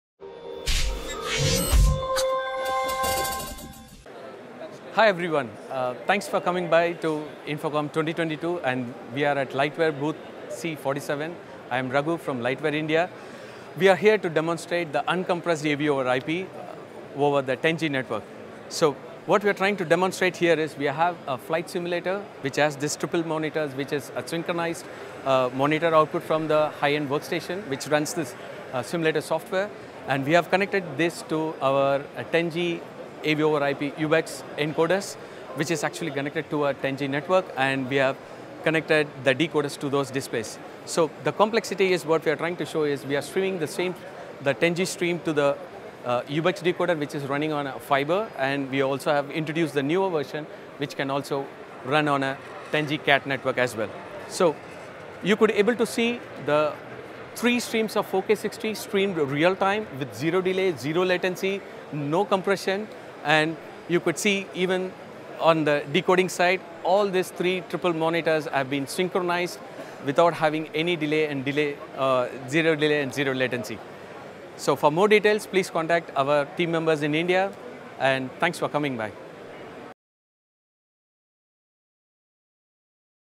Flight simulator demonstrates Lightware capabilities at InfoComm India 2022
Raguvaran Patel shows us how a flight simulator demonstrates the capabilities of Lightware's signal transmission capabilities over IP.